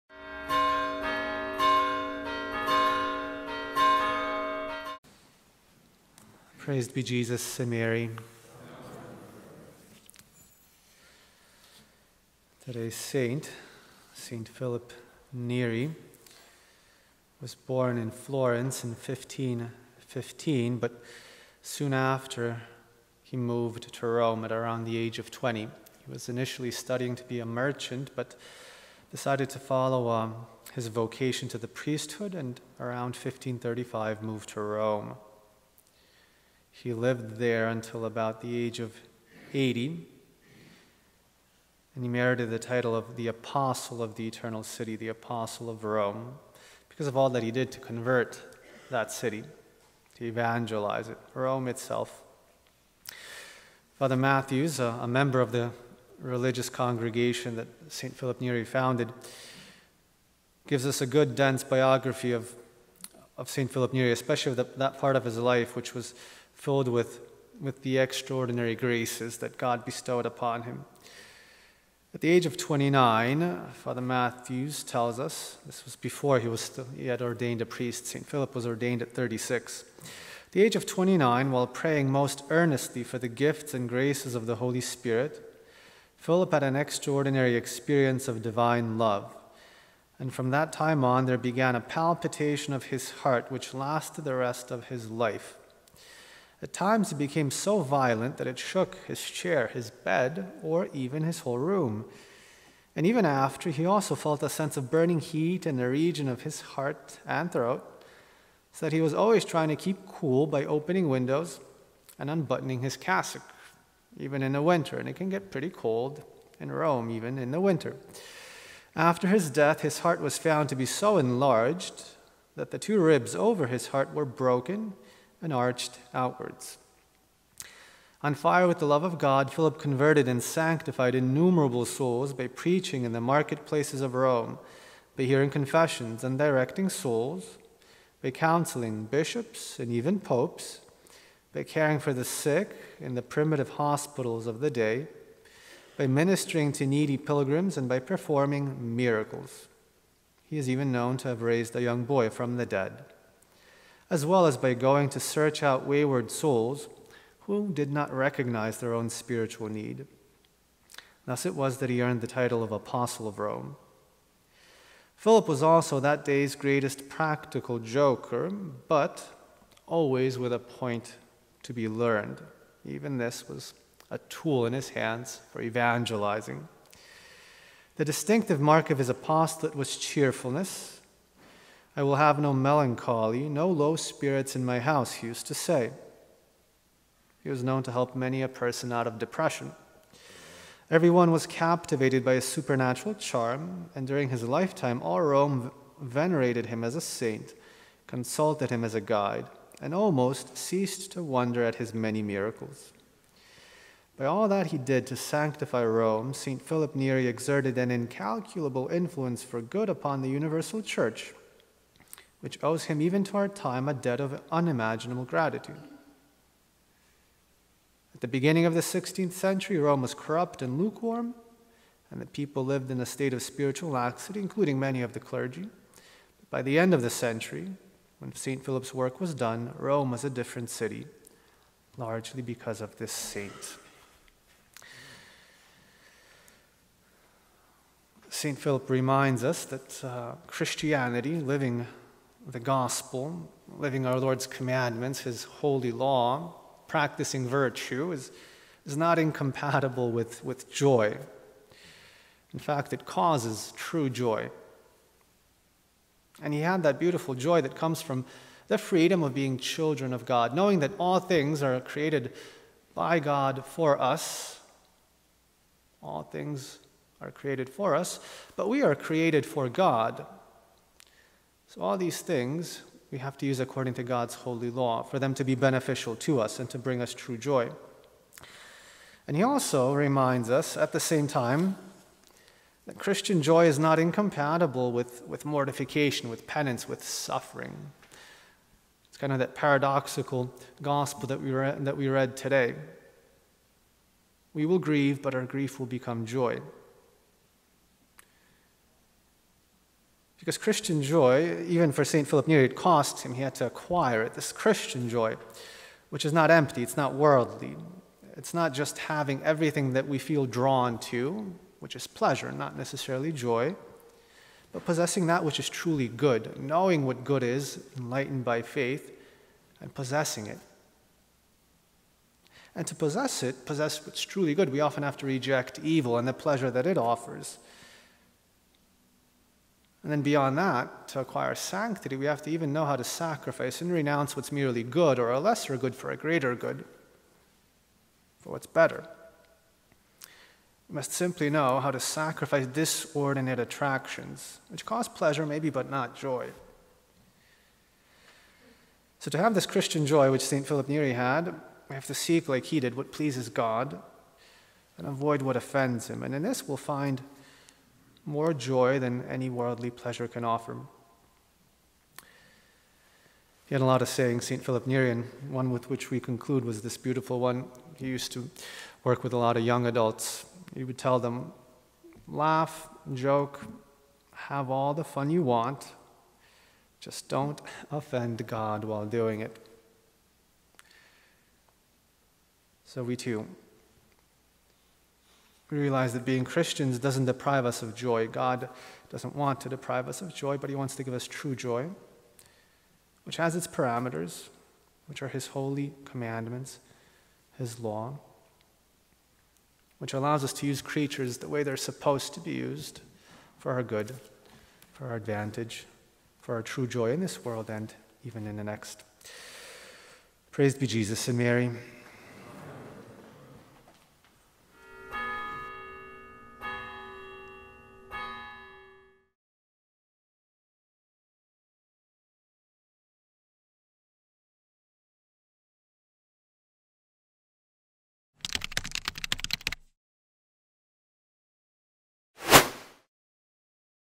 May 26 - Homily: Fiery Humor of St. Phillip Neri
Fr. Matthias on the life of St. Phillip Neri and his unique relation to God where he felt an incredible fire of love in his heart combined with a great sense of humor and was so effective at evangelizing Rome.

Ave Maria!

Mass: St. Philip Neri - Mem
Readings: Friday 6th Week of Easter
1st: act 18:9-18
Resp: psa 47:2-3, 4-5, 6-7 0
Gsp: joh 16:20-23
For mp3 Audio of this video, go to - AirMaria.com